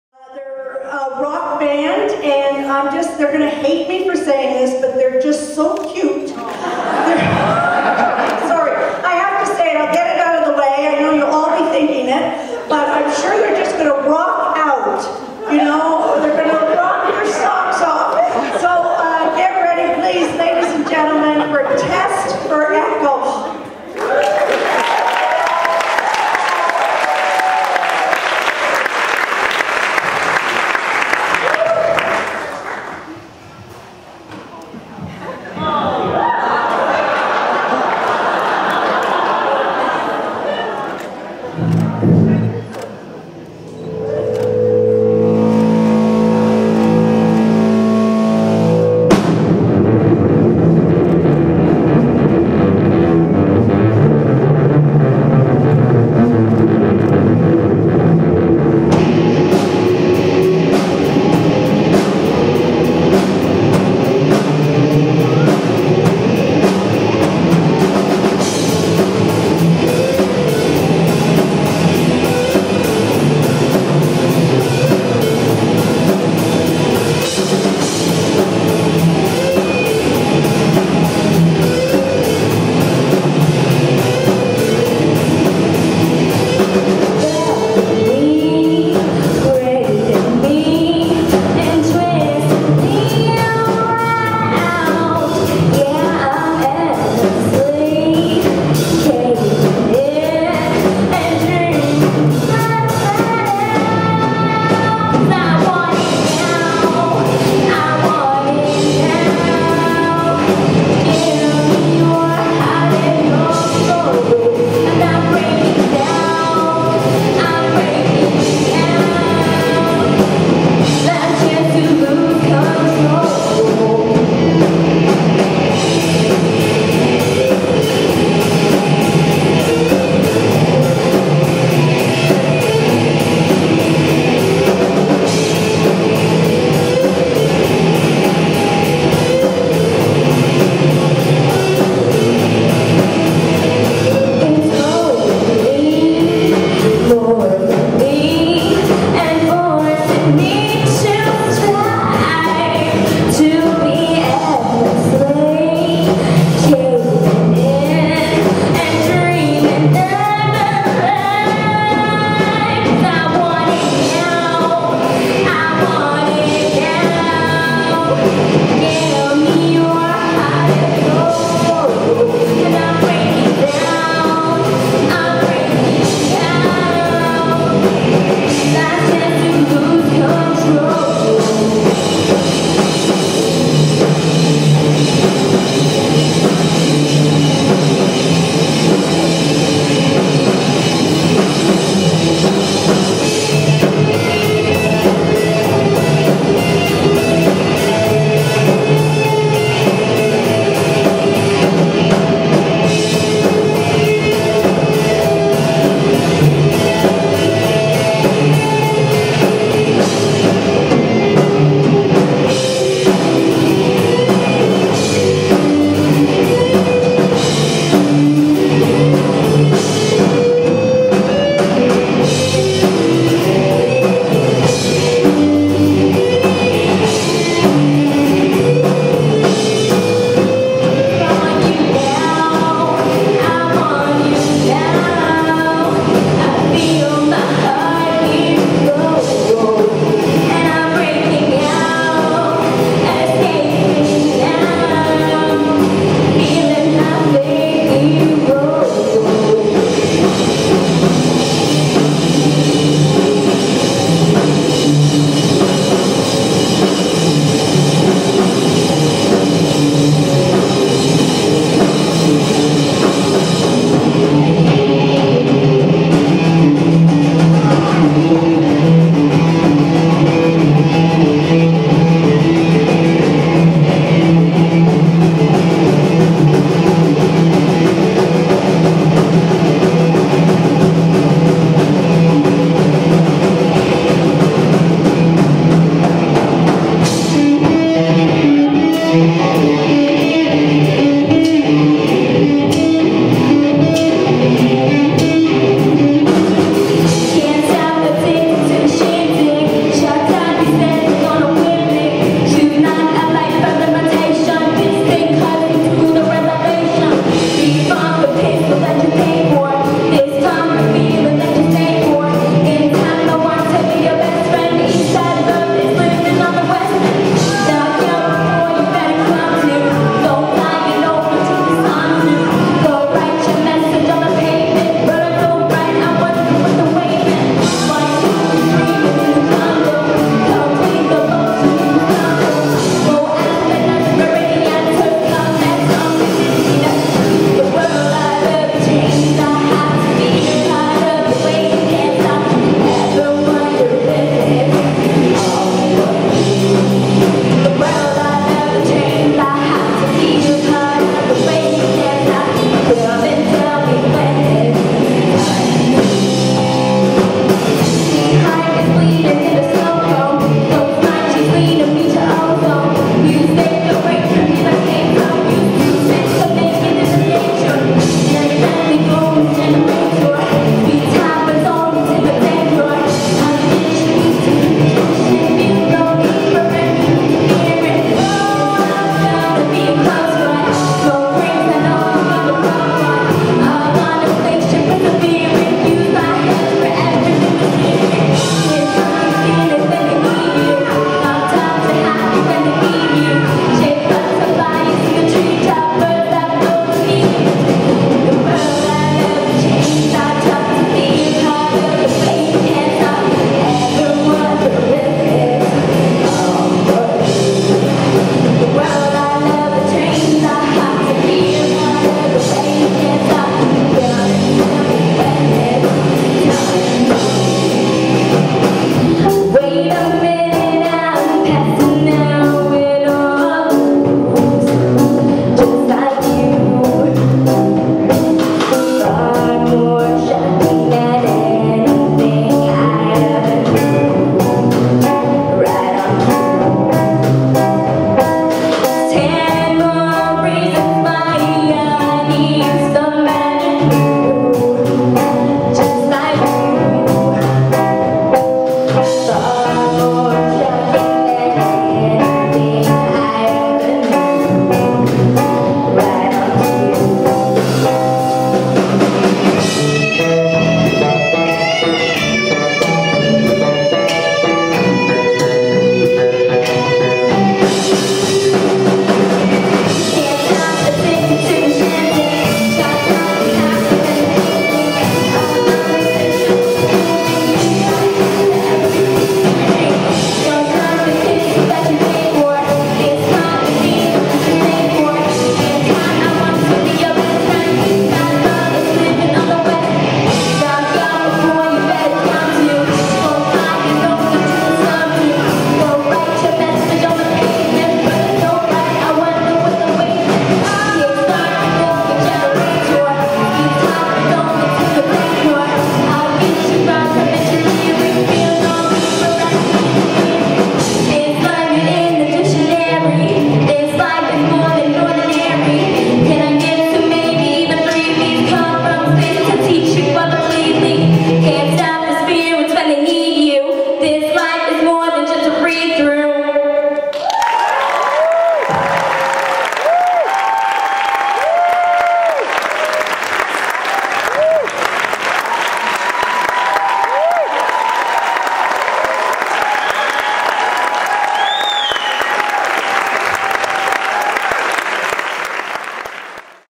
Test For Echo HD
Test For Echo at Lakeshore Arts Showcase June 16, 2012. Hilarious introduction by former SCTV and SNL comedian, Robyn Duke . Band performed as one of the opening acts for Canadian Maple Blues Female Vocalist of the Year, Suzie Vinnick.

A great evening of entertainment!!!